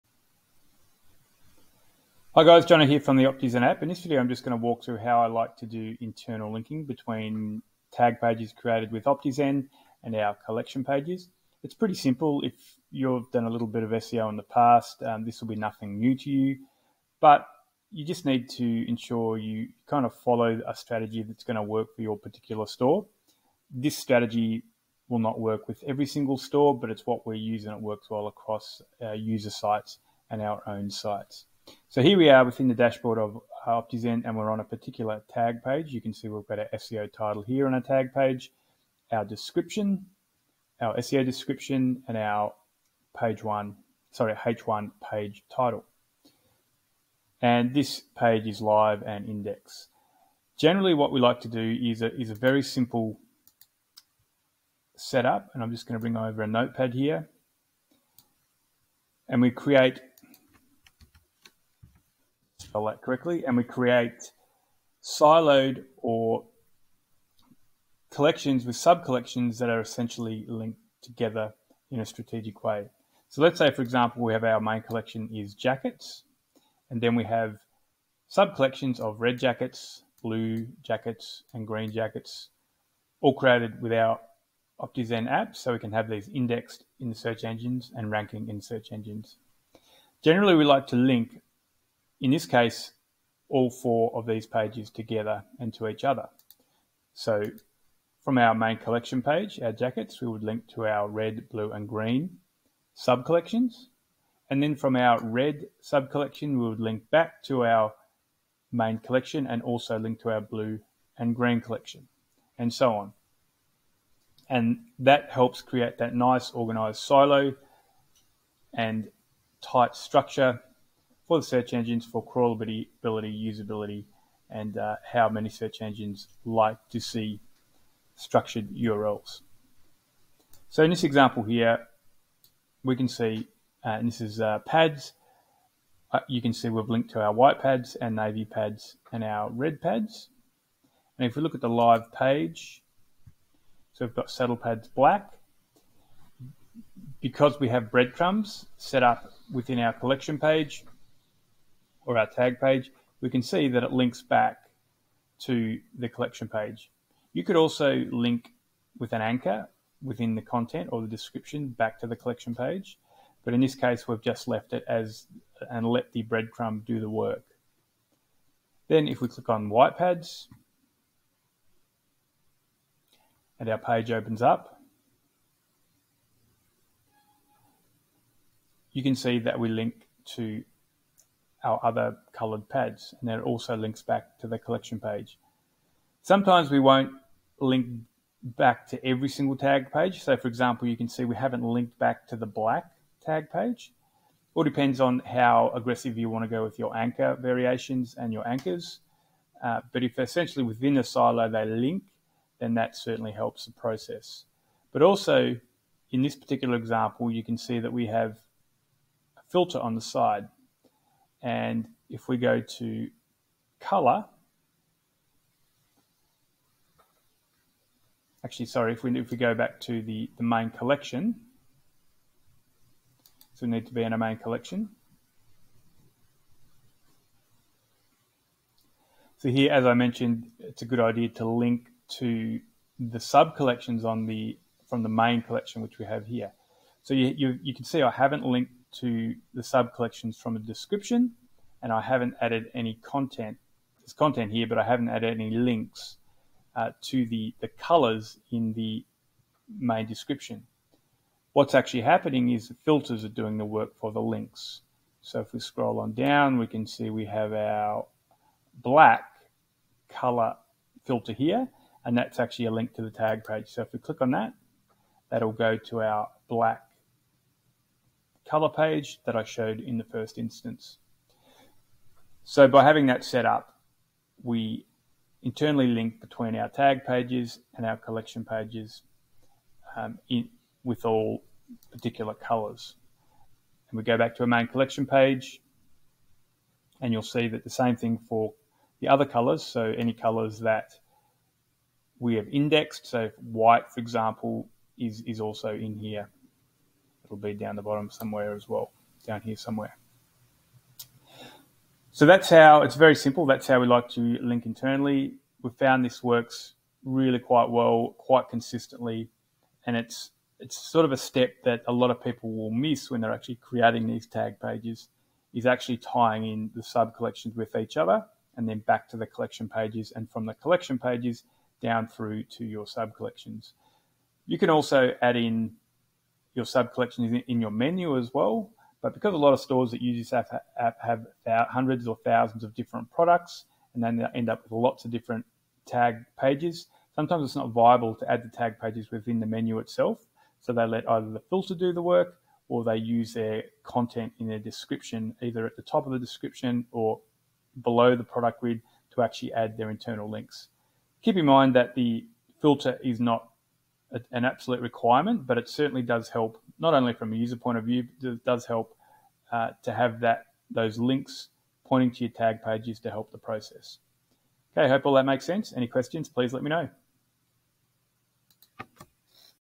Internal Linking Using Optizen - Shopify SE
In this video I walk through how we do simple internal linking using the Optizen app on tag pages and collections pages.